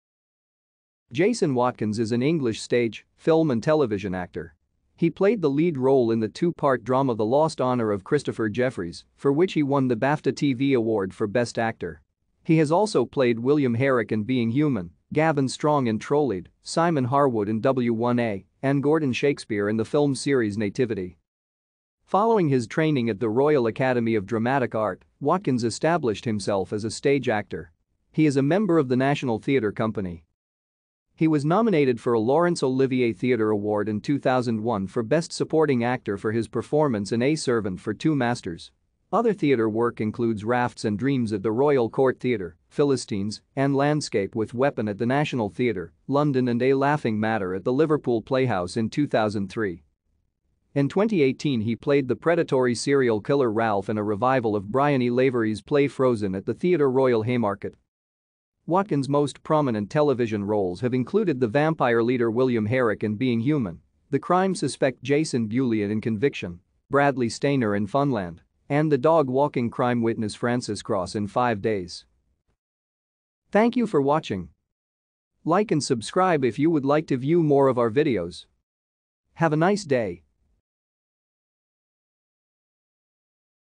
Jason Watkins (Actor)
Jason Watkins  is an English stage,  and  actor.  He played the lead role in the two-part drama The Lost Honour of Christopher Jefferies, for which he won the BAFTA TV Award for Best Actor. He has also played William Herrick in Being Human, Gavin Strong in Trollied, Simon Harwood in W1A, and Gordon Shakespeare in the   Nativity.